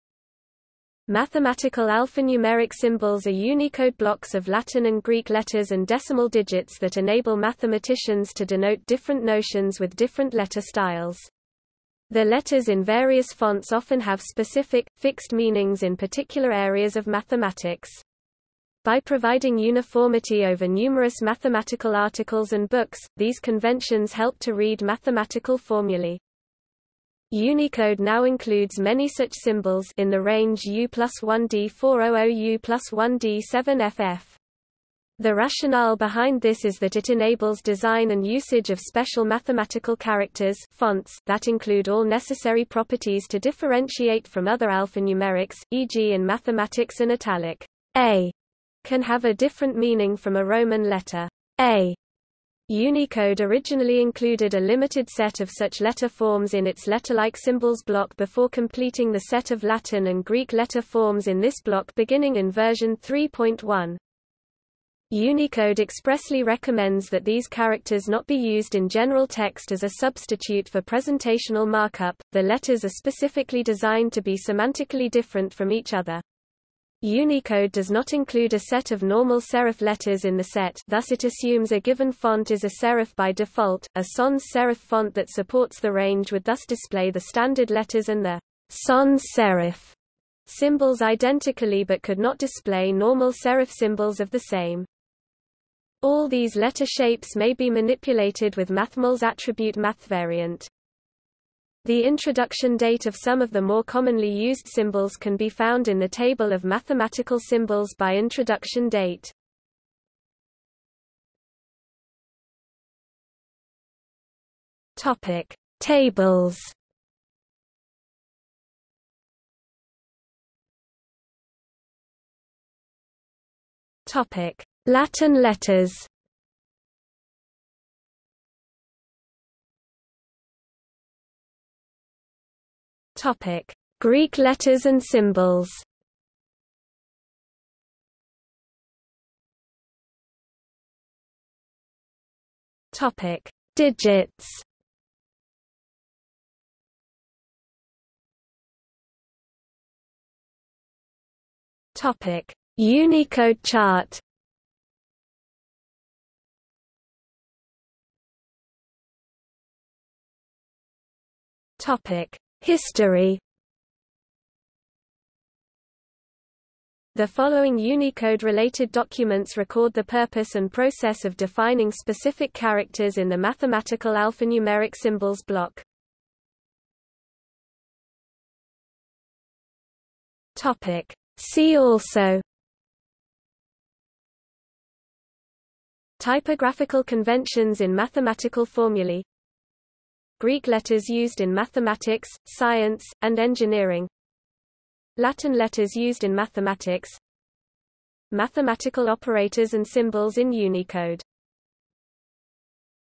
Mathematical Alphanumeric Symbols | Wikipedia audio article
00:01:51 1 Tables
00:02:00 1.1 Latin letters
00:02:09 1.2 Greek letters and symbols
00:02:19 1.3 Digits
00:02:28 2 Unicode chart
00:02:38 3 History
00:02:56 4 See also

- Socrates

SUMMARY
Mathematical Alphanumeric Symbols are Unicode blocks of Latin and Greek letters and decimal digits that enable mathematicians to denote different notions with different letter styles. The letters in various fonts often have specific, fixed meanings in particular areas of mathematics. By providing uniformity over numerous mathematical articles and books, these conventions help to read mathematical formulae.
Unicode now includes many such symbols (in the range U+1D400–U+1D7FF).  The rationale behind this is that it enables design and usage of special mathematical characters (fonts) that include all necessary properties to differentiate from other alphanumerics, e.g. in mathematics an italic "A" can have a different meaning from a roman letter "A". Unicode originally included a limited set of such letter forms in its Letterlike Symbols block before completing the set of Latin and Greek letter forms in this block beginning in version 3.1.
Unicode expressly recommends that these characters not be used in general text as a substitute for presentational markup; the letters are specifically designed to be semantically different from each other. Unicode does not include a set of normal serif letters in the set (thus it assumes a given font is a serif by default; a sans-serif font that supports the range would thus display the standard letters and the "sans-serif" symbols identically but could not display normal serif symbols of the same).
All these letter shapes may be manipulated with MathML's attribute mathvariant.
The introduction date of some of the more commonly used symbols can be found in the Table of mathematical symbols by introduction date.